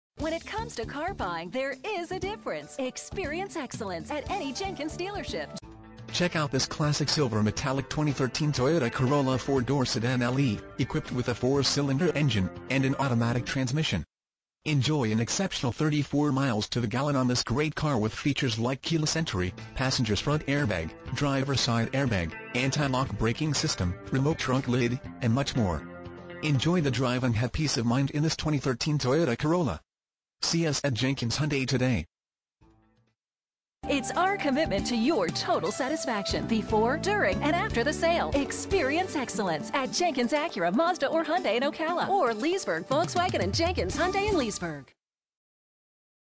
2013 Toyota Corolla JENKINS HYUNDAI Ocala, FL 34474
Toyota Corolla Check out this  Classic Silver Metallic   Toyota Corolla 4 Dr Sedan, equipped with a 4 Cylinder engine and an automatic transmission. Enjoy an exceptional 34 miles to the gallon on this great car with features like Keyless Entry, Passenger's Front Airbag, Driver Side Air Bag, Anti-Lock Braking System, Remote Trunk Lid, and much more. Enjoy the drive and have peace of mind in this  Toyota Corolla. See us at JENKINS HYUNDAI today!
MILES: 32,790
VIN:5YFBU4EE5DP096177

JENKINS HYUNDAI
1602 SW College Rd.